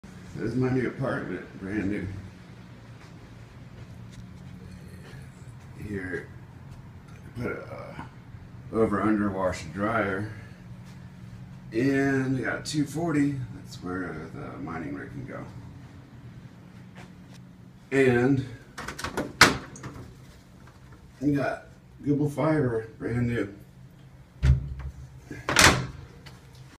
Stealth laundry room mining rig setup (part 1 of ??)
Has 220V outlet and plenty of room as long as I get an over-under washer dryer ($25/month to rent). Also has fiber optic which is rare in this area. The electric is submetered from building's commercial account so should be more affordable than residential rates.

Edit: I was wondering on about what to do with the excess heat and thought about cutting a hole in the back wall so it get's sucked into the AC system, however, I have a dryer vent right there so why not just use that?  I've already seen a quick-connect system for dryer hoses on another youtube howto video.
BTW: the filter is just for fun.  I'll put in a link the original version when I get a sec to find it;